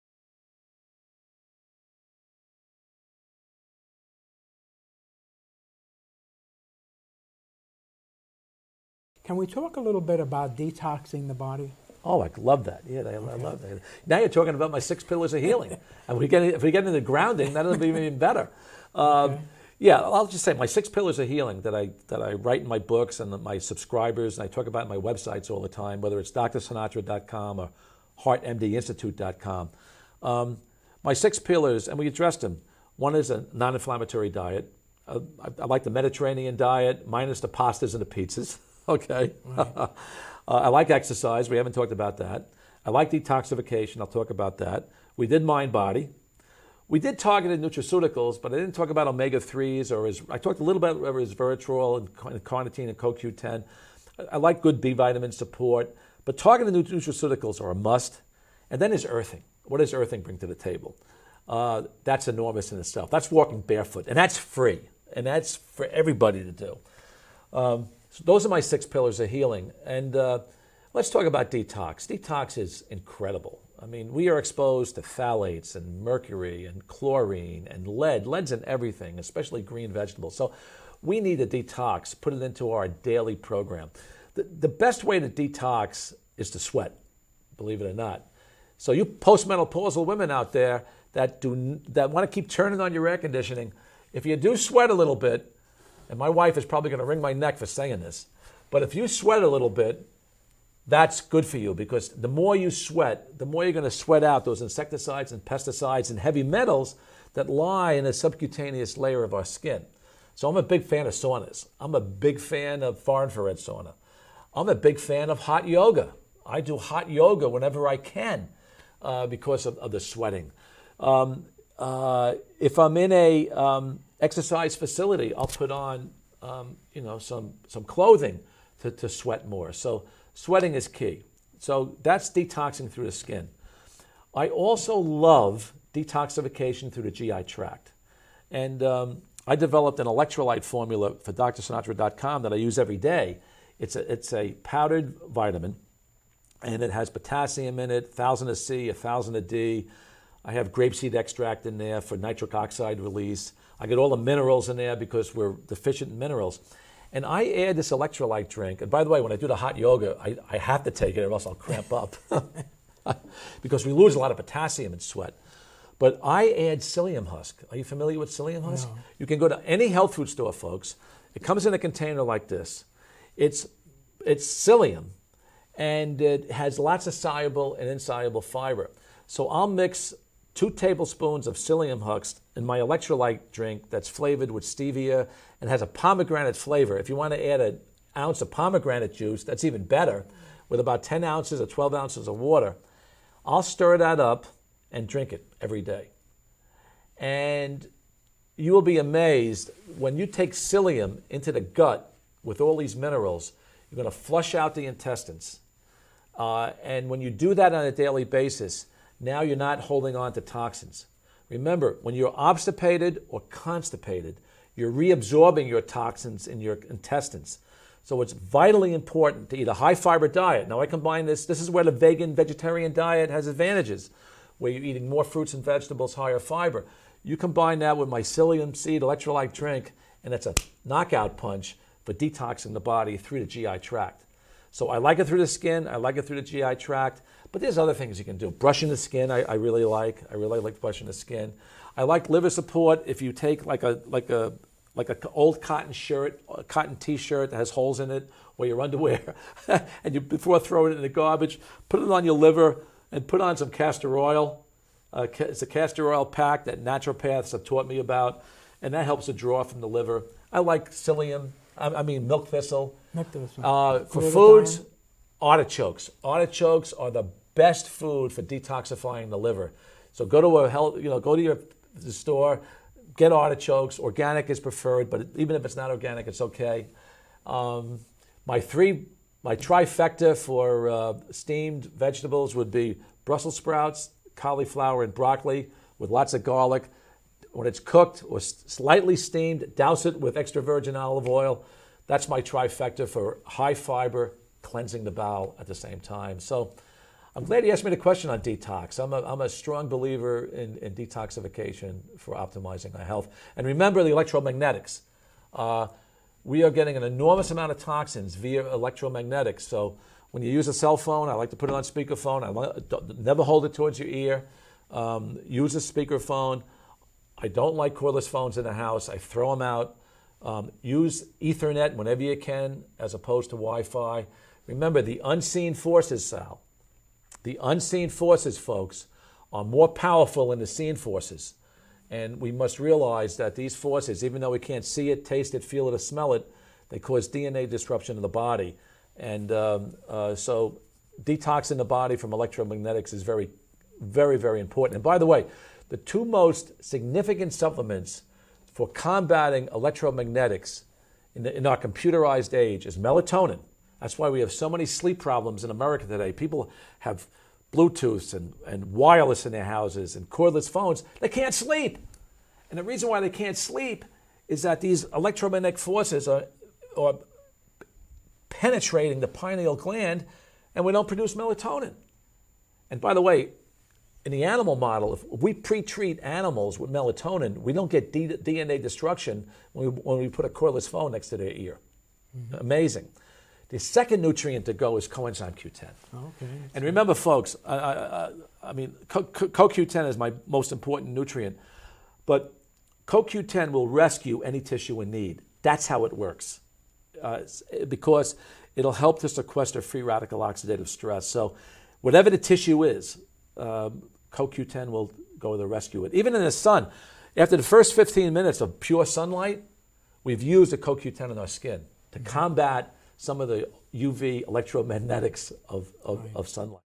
Dr Sinatra MD. Benefits of Detoxing the Body.
Dr. Sinatra The Benefits of Detoxing the Body.
TV Interview.